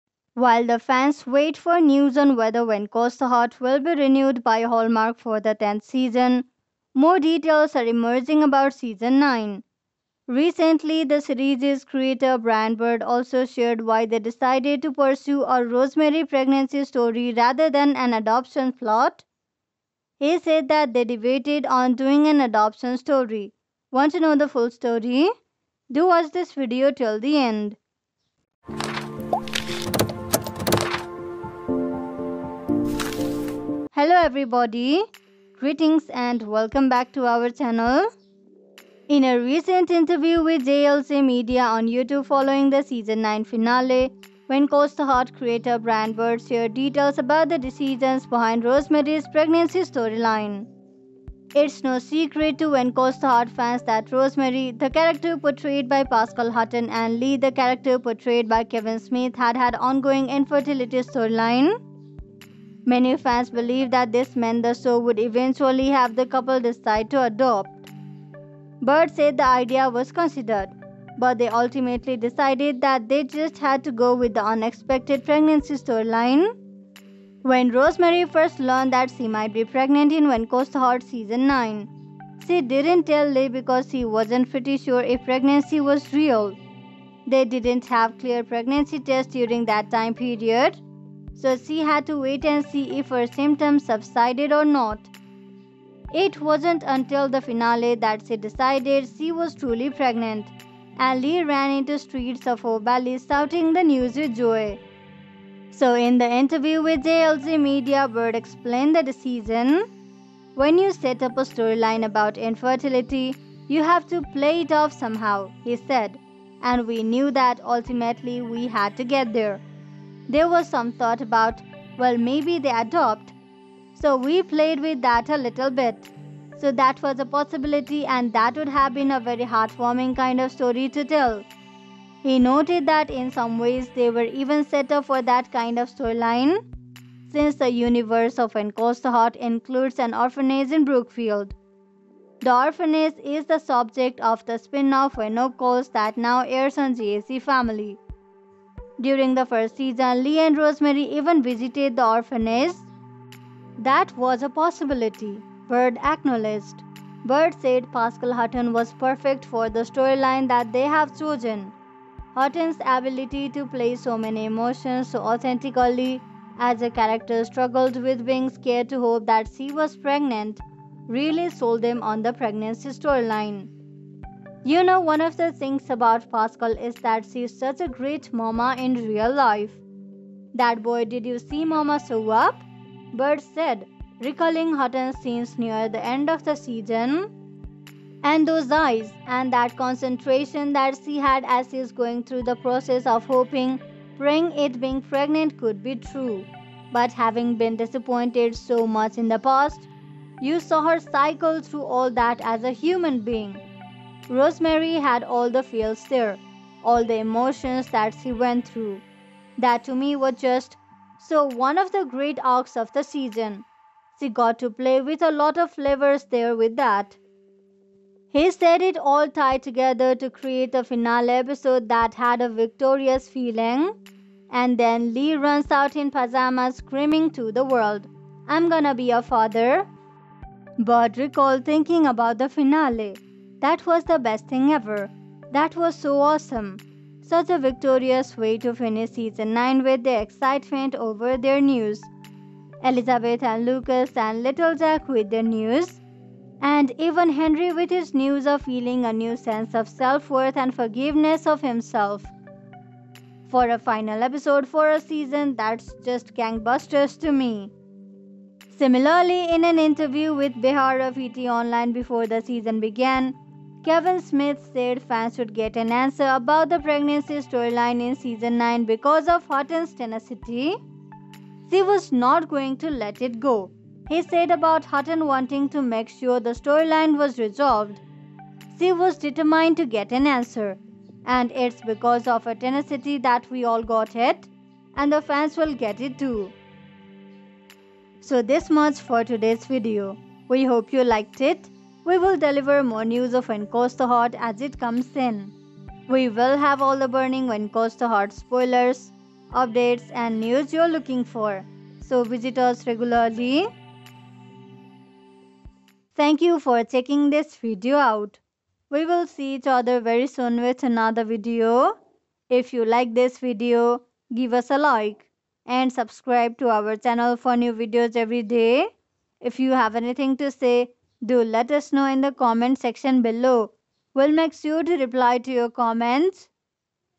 When Calls the Heart Season 10 News: Perfect Storyline for Lee & Rosemary
While the fans wait for news on whether “When Calls the Heart” will be renewed by Hallmark for the tenth season, more details are emerging about season 9. Recently, the series’ creator, Brian Bird, also shared why they decided to pursue a Rosemary pregnancy story rather than an adoption plot. He said that they debated on doing an adoption story. Want to know the full story? Do watch this video till the end.

Hello Everybody. In a recent interview with JLJ Media on YouTube following the season 9 finale, When Calls The Heart creator Brian Bird shared details about the decisions behind Rosemary’s pregnancy storyline. It’s no secret to “When Calls the Heart” fans that Rosemary, the character portrayed by Pascale Hutton and Lee, the character portrayed by Kavan Smith have had an ongoing infertility storyline. Many fans believed that this meant the show would eventually have the couple decide to adopt. Bird said the idea was considered, but they ultimately decided that they just had to go with the unexpected pregnancy storyline.